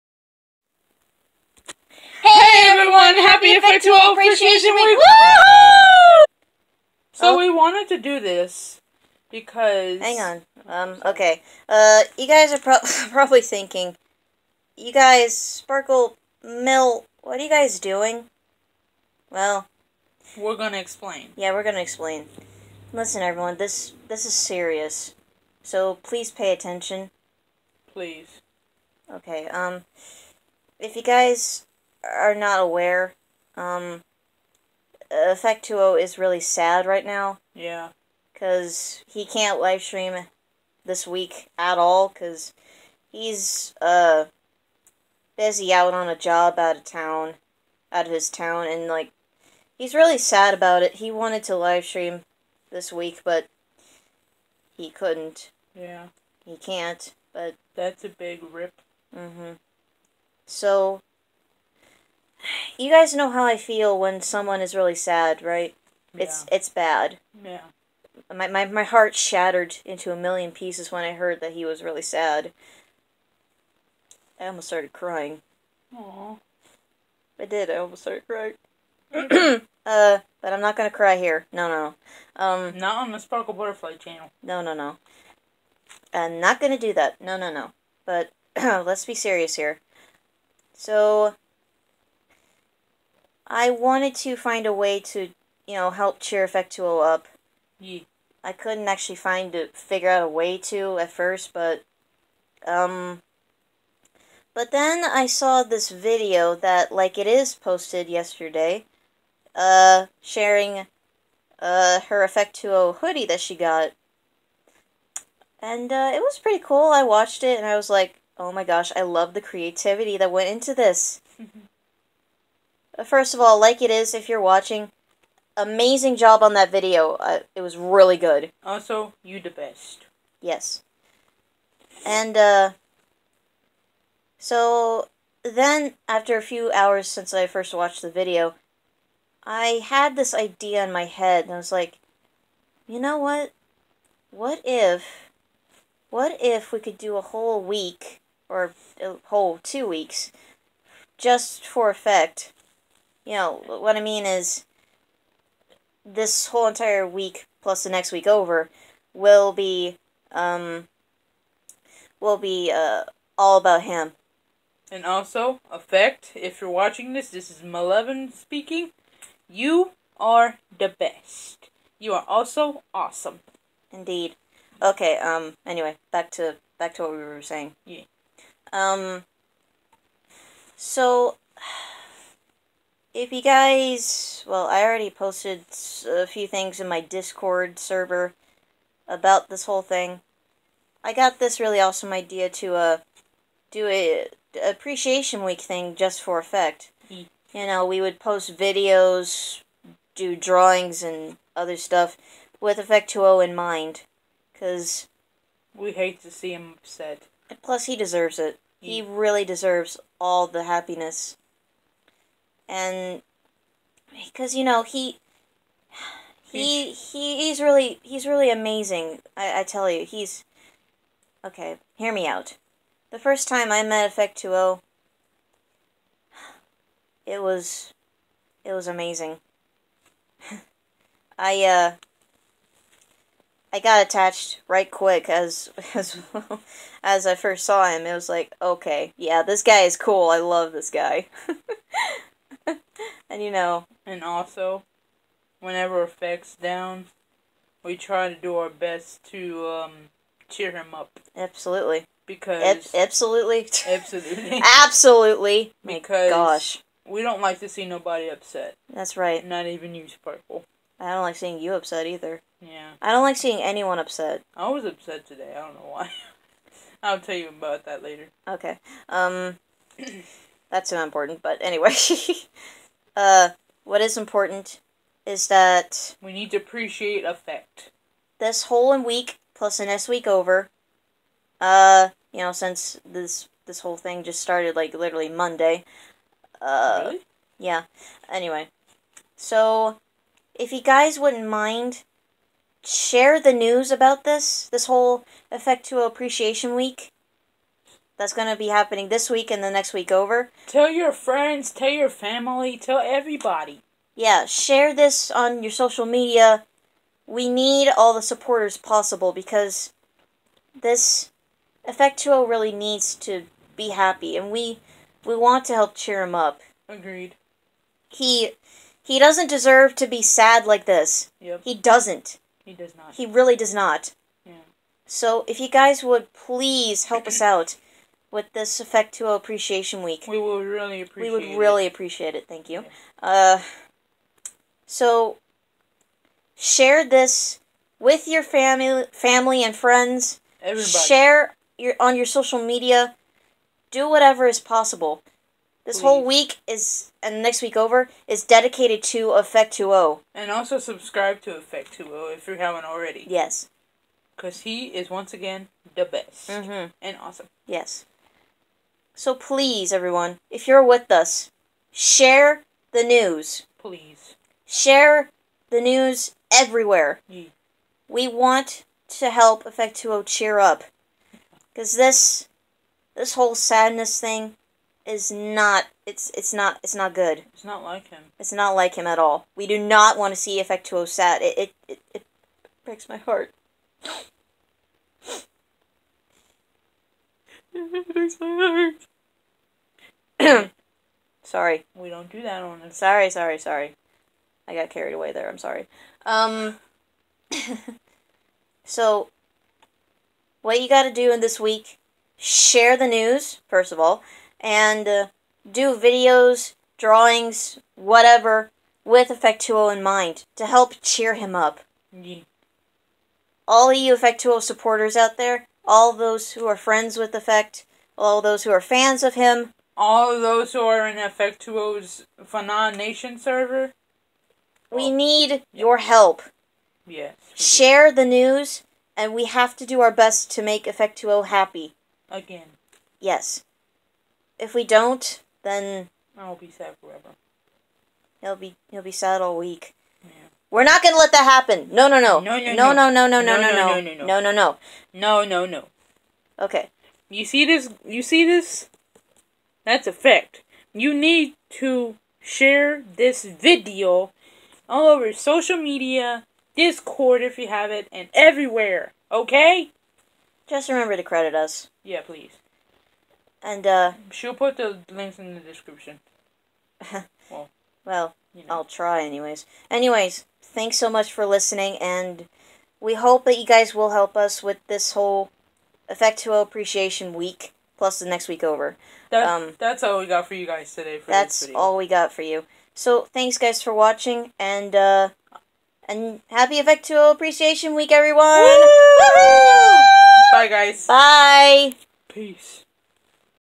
Important: Effect2o Appreciation Week!! Share The News And Join Us!!
Listen everyone! This is serious!! We need your help!!

Effect2o is really really sad right now, cause he can't livestream this week cause he has a job that is out of his town.

This is unacceptable, because.. Effect doesn't deserve to be sad!! He really doesn't!! So, I came up with the idea of giving him his own Appreciation Week for this whole week plus the next week, cause this literally started Monday, but I never said much about it.

Anyways, we need your help with this!! We need you to share the news about this week and the next week and we would really love your support with this! This week plus next week will be all about Effect2o!! We will do videos, drawings, etc. with Effect in mind to help cheer him up!! Please help us!! We need Effect to be happy!! He's a really wonderful person and he has done a lot to make us happy! Now, it's our turn to do the same!! Please join us!!